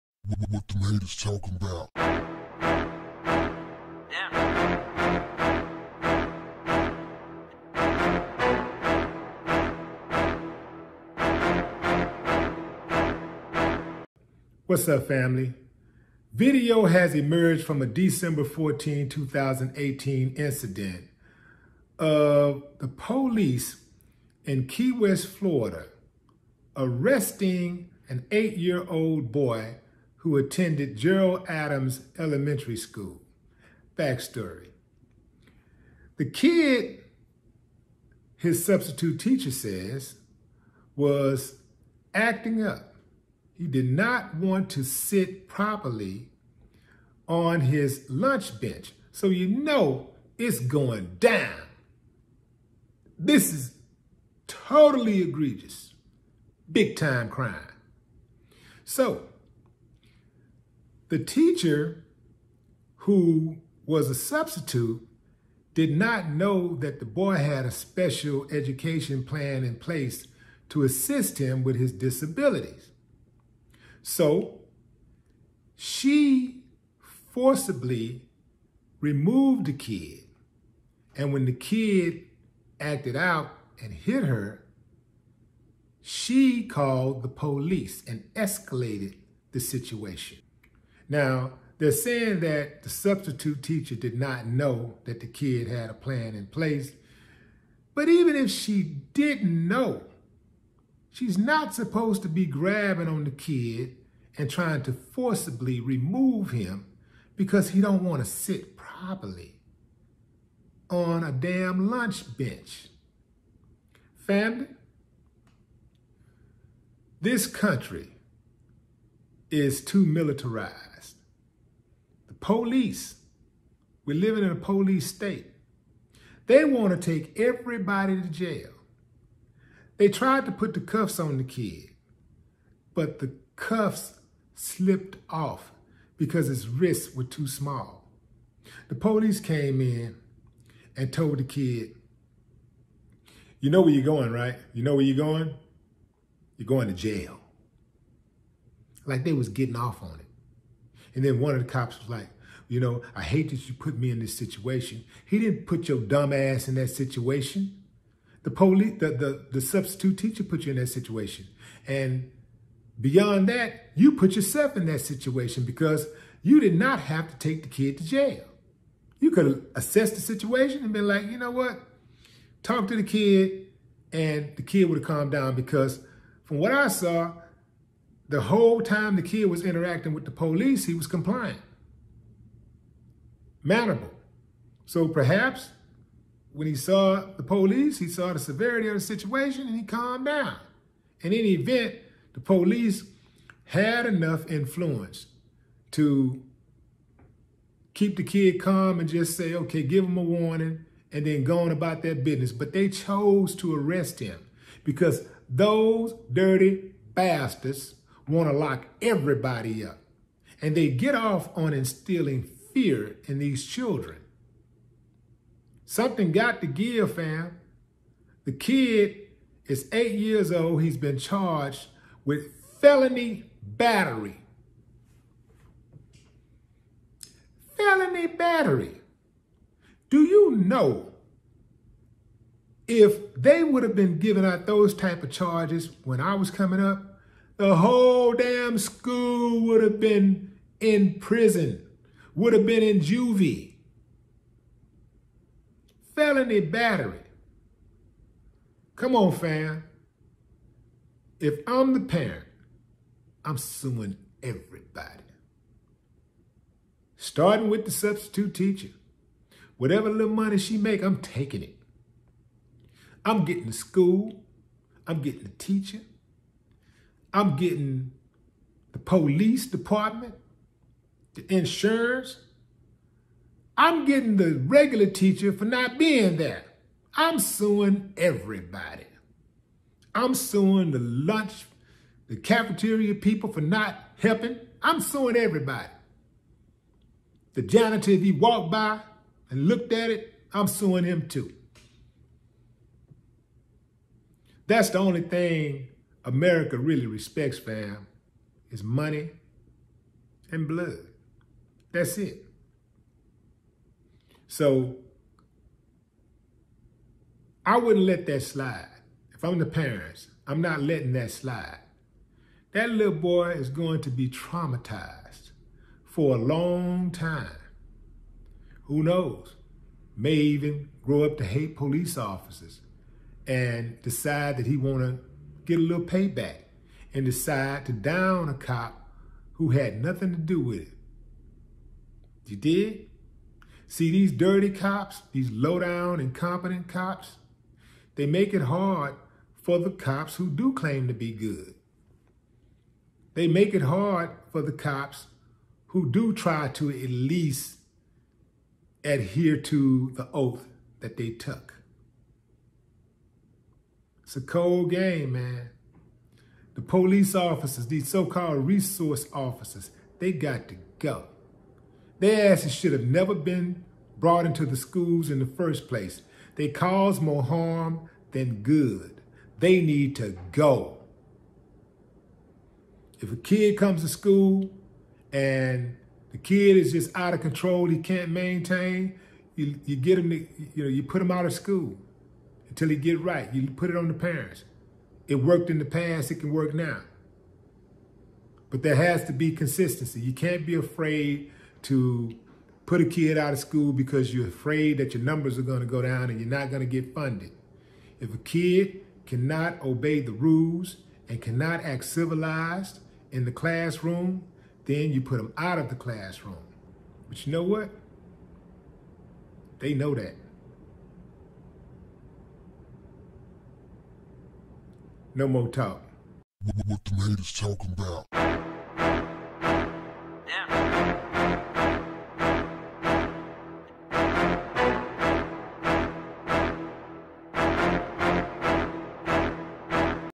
Video Shows Florida Police Struggle To Handcuff 8-Year Old Boy
Attention Subs!!!

$WillieDLive

$WillieDLive